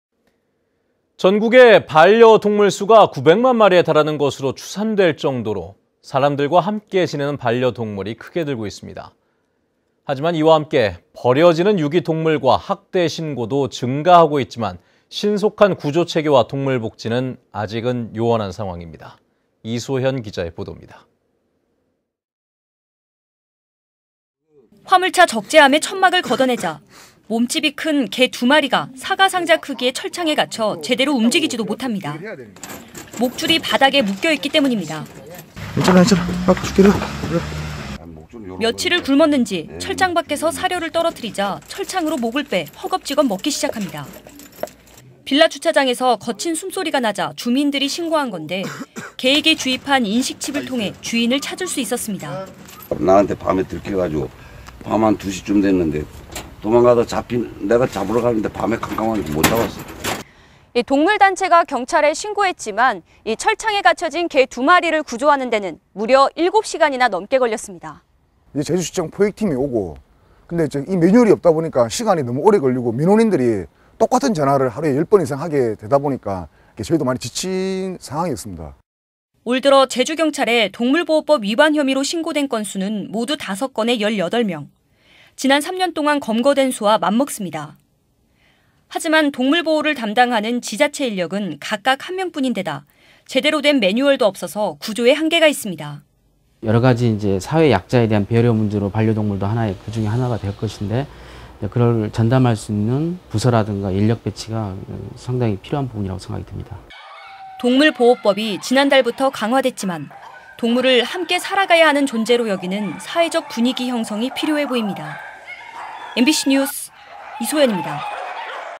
미흡한 구조체계 갈길 먼 동물복지 이소현 강흥주
제주MBC 리포트 미흡한 구조체계   갈길 먼 동물복지 이소현

           ◀ＡＮＣ▶
 전국에 반려 동물 수가 900만 마리로
추산될 정도로
사람들과 함께 지내는 반려 동물이
크게 늘고 있습니다.

 하지만 버려지는 유기 동물과
학대 신고도 증가하고 있지만
신속한 구조체계와 동물복지는
요원한 상황입니다.

           ◀ＥＮＤ▶
           ◀ＶＣＲ▶

 화물차 적재함의 천막을 걷어내자,

 몸집이 큰 개 2마리가
사과 상자 크기의 철창에 갇혀
제대로 움직이지도 못합니다.

 목줄이 바닥에 묶여 있기 때문입니다.

◀ＳＹＮ▶ 동물단체
"괜찮아, 괜찮아, 구해줄게."

 며칠을 굶었는지
철창 밖에서 사료를 떨어뜨리자
철창으로 목을 빼내
허겁지겁 먹기 시작합니다.

 빌라 주차장에서
거친 숨소리가 나자 주민들이 신고한건데,
개에게 주입한 인식칩을 통해
주인을 찾을 수 있었습니다.

◀ＳＹＮ▶ 견주
"(열흘 전) 나한테 (절도범이) 밤에 들켜서. 밤 2시쯤 됐는데. 내가 잡으러 갔는데 캄캄해서 못 잡았어요."

 "(s.u) 동물단체가 경찰에 신고했지만
철창에 갇혀진 개 2마리를 구조하는데는
무려 7시간이나 넘게 걸렸습니다."

 ◀ＩＮＴ▶ 윤동선
            부산동물학대 방지연합 제주팀장
"(경찰도 오고) 제주시청 포획팀도 오고. 그런데 매뉴얼이 없다 보니까 시간이 너무 오래 걸리고, 민원인들이 똑같은 전화를 하루에 10번 이상 하게 되니까 저희도 많이 지친 상황이었습니다."

 (c.g) 올 들어 제주 경찰에
동물보호법 위반 혐의로 신고된 건수는
모두 5건에 18명.

 지난 3년동안 검거된 수와 맞먹습니다. /

 하지만 동물 보호를 담당하는
지자체 인력은 각각 1명 뿐인데다
제대로운 매뉴얼도 없어서
구조에 한계가 있습니다.

◀ＩＮＴ▶ 윤영민 제주대학교 수의과대학 교수
"여러 가지 사회 약자에 대한 배려 문제로 반려동물도 그중 하나가 될 것인데 전담할 수 있는 부서나 인력 배치가 상당히 필요합니다."

 동물보호법이 지난달부터 강화됐지만
동물을 함께 살아가야 하는 존재로 여기는
사회적 분위기 형성이 필요해보입니다.